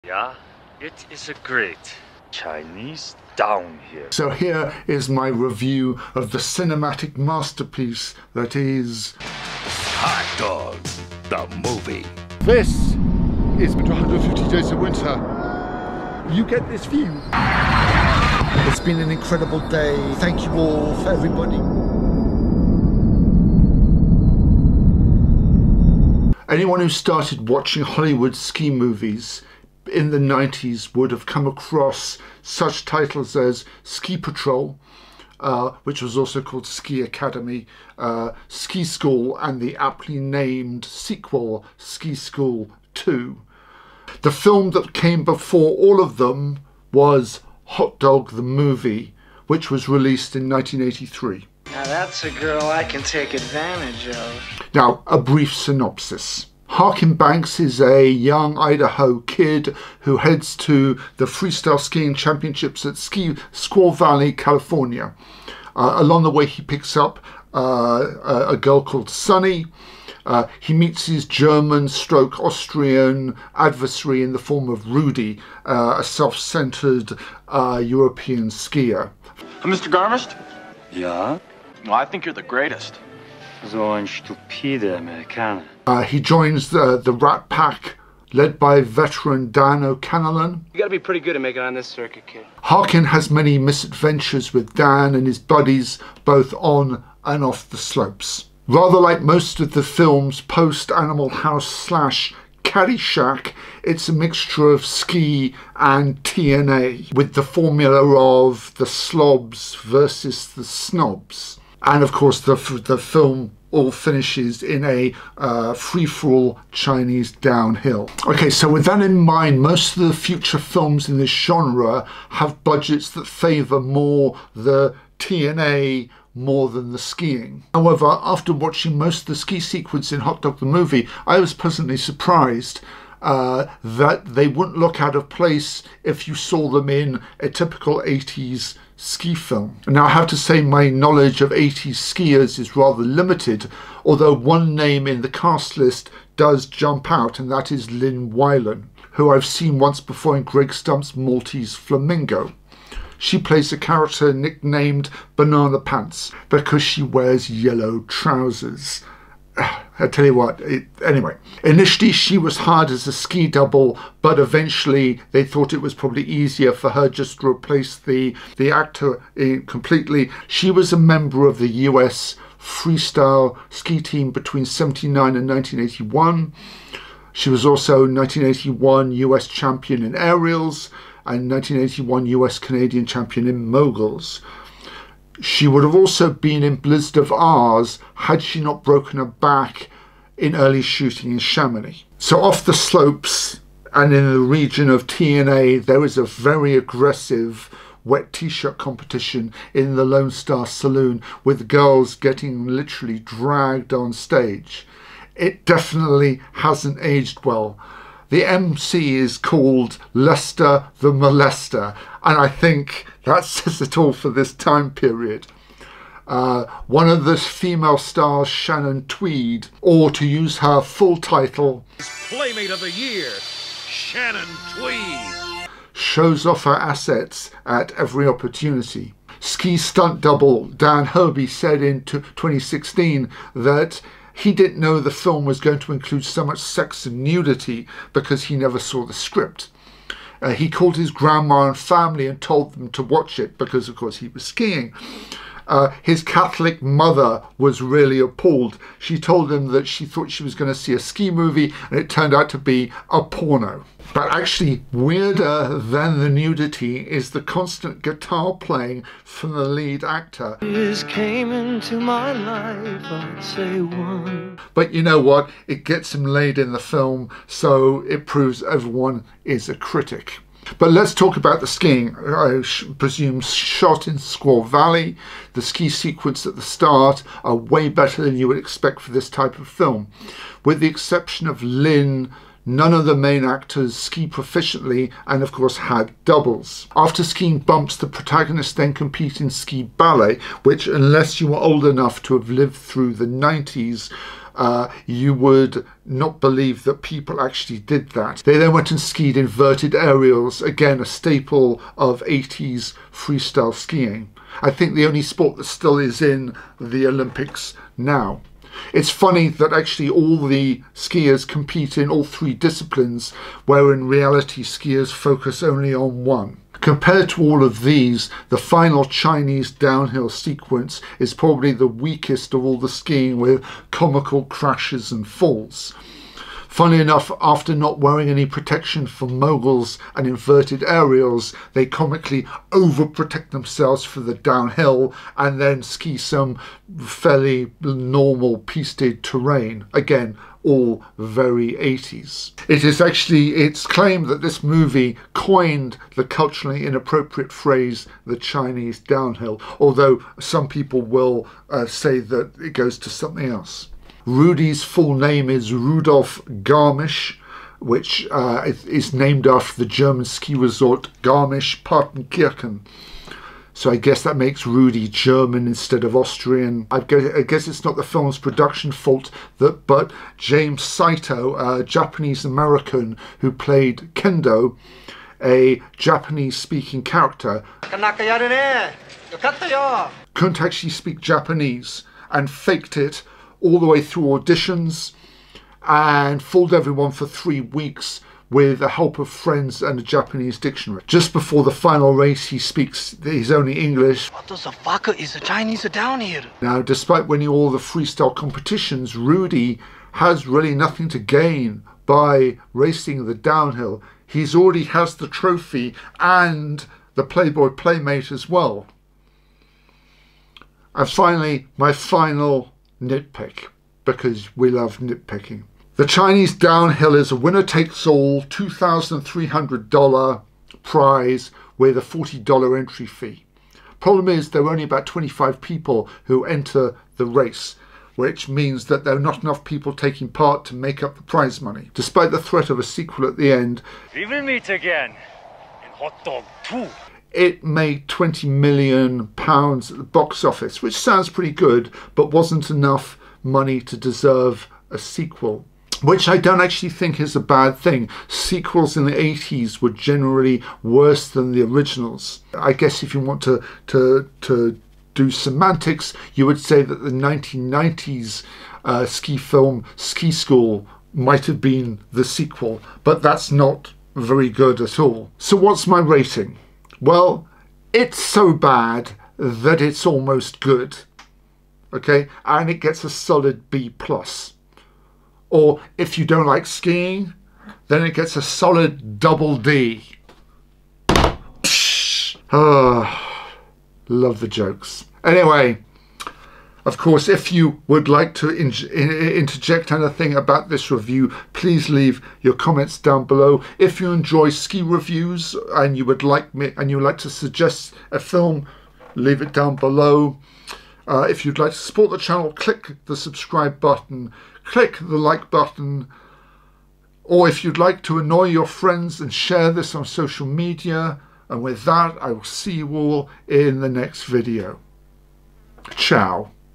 HOT DOG THE MOVIE (1983) SKI MOVIE REVIEW
One of the first 80's Hollywood ski films Hot Dog the movie has become a classic of how low-budget films used to be made. Using the age-old formula of the Snobs vs the Slobs an American takes on a German skier in a freestyle competition culminating in a Chinese downhill. Love it or hate it.